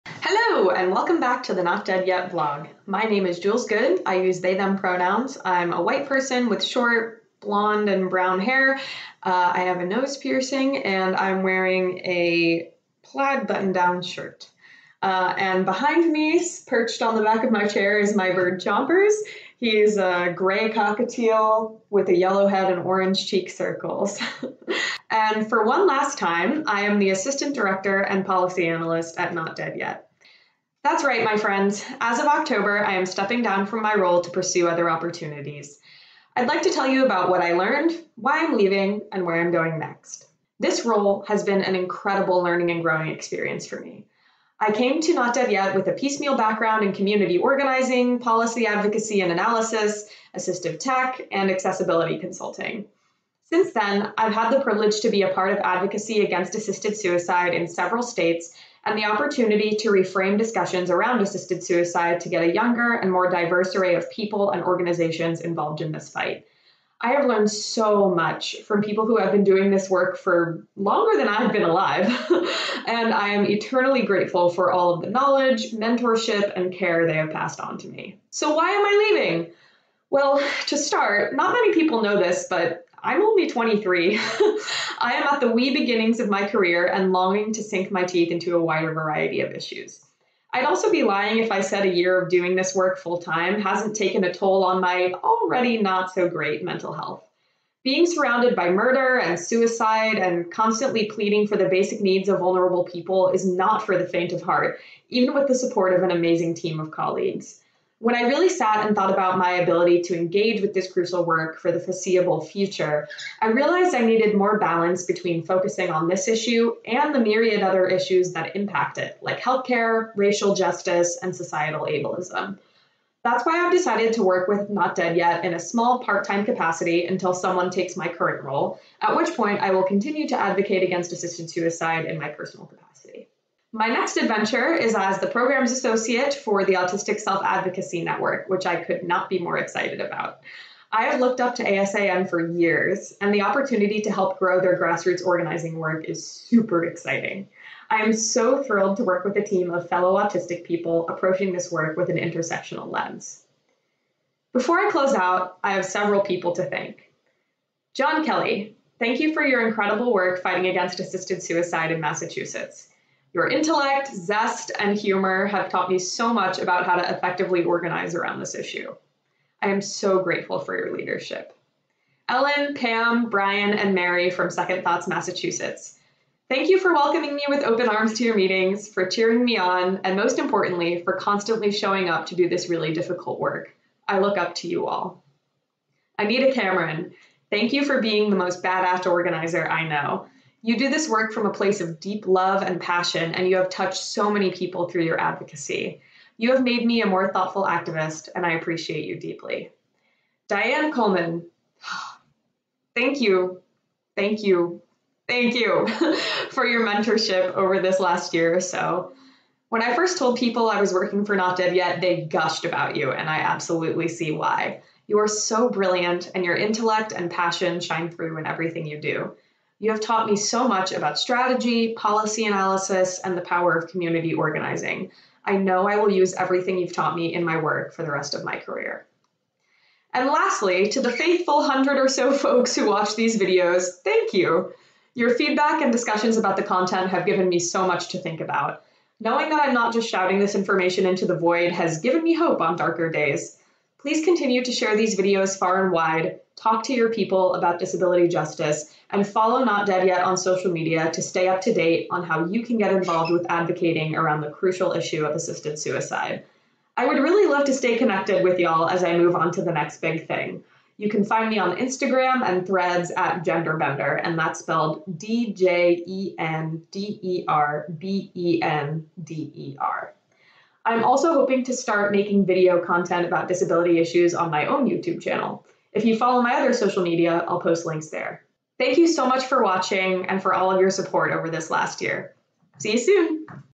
Farewell, but not Goodbye!
I'm onto the next adventure! A quick vid to let you know why I'm leaving my current position at NDY, what I've learned, and where I'm going next.